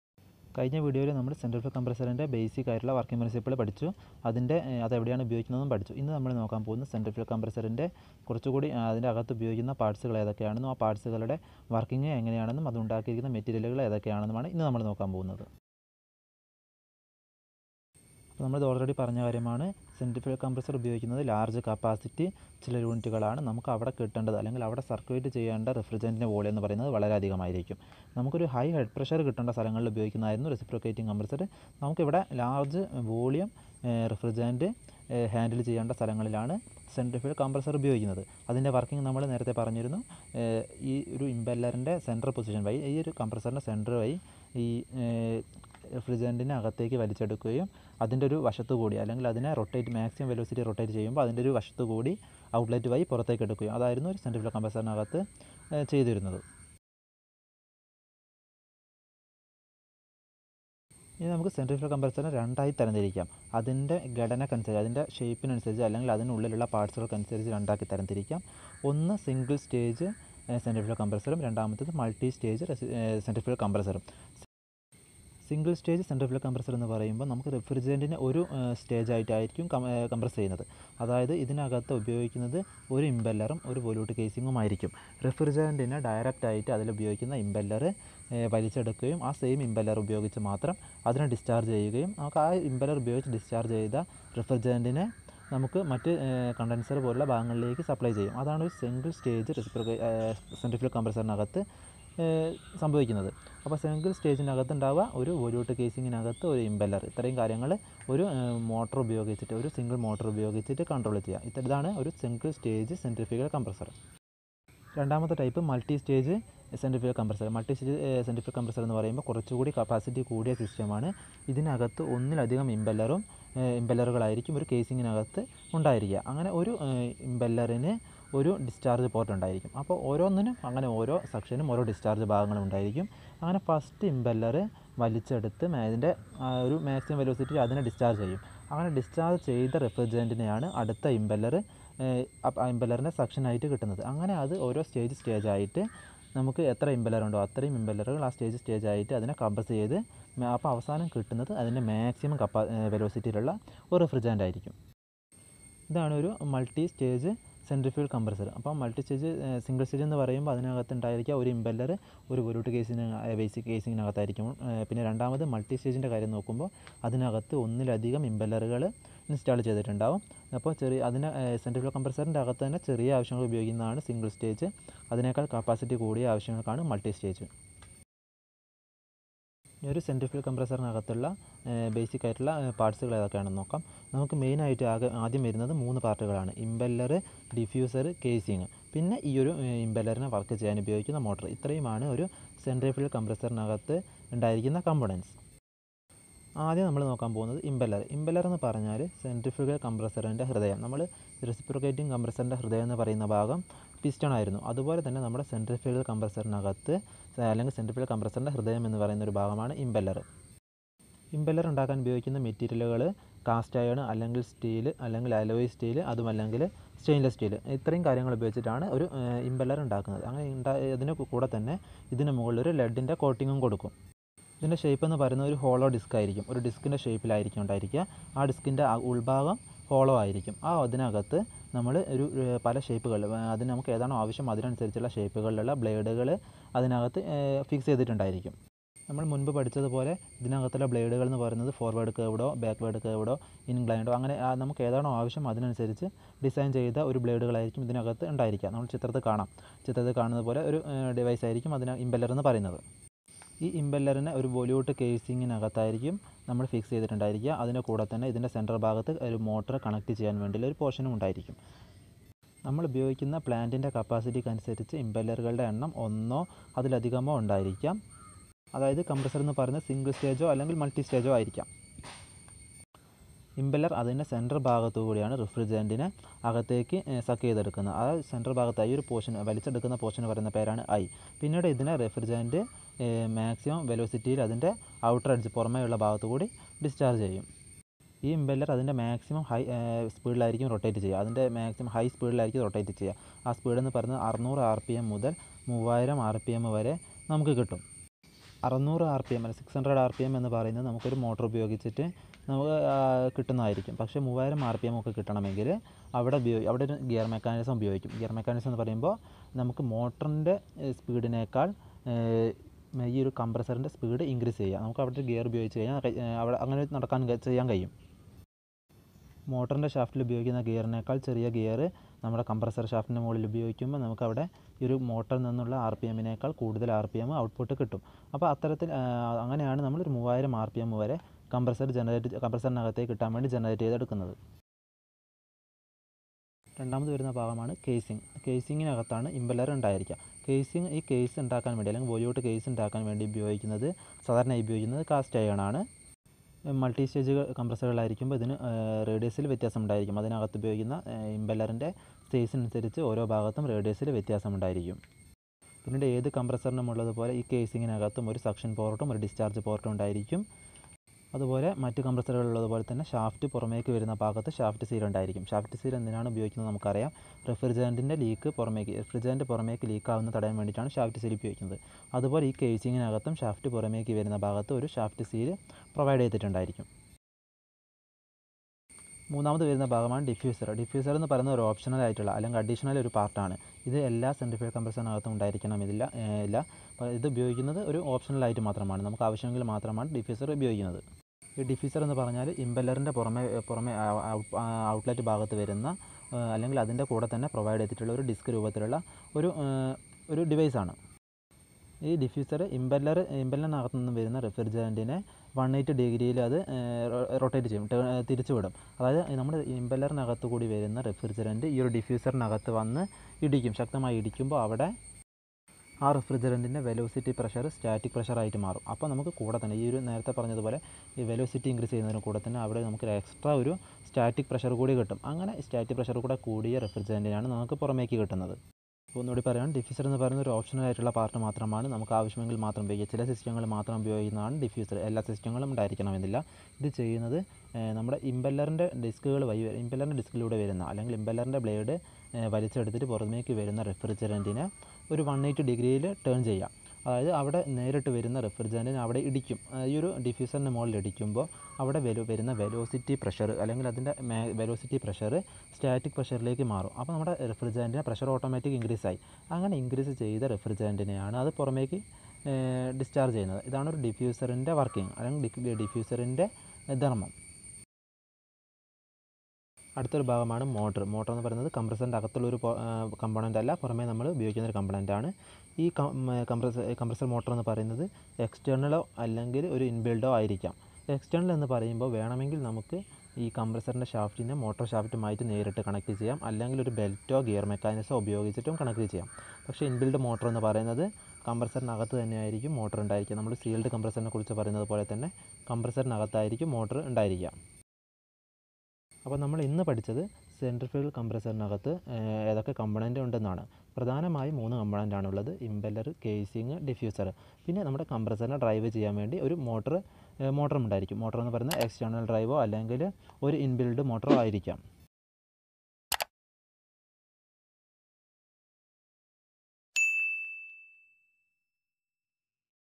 Centrifugal compressor part 2 malayalam
Online class for iti ncvt MRAC trainees in kerala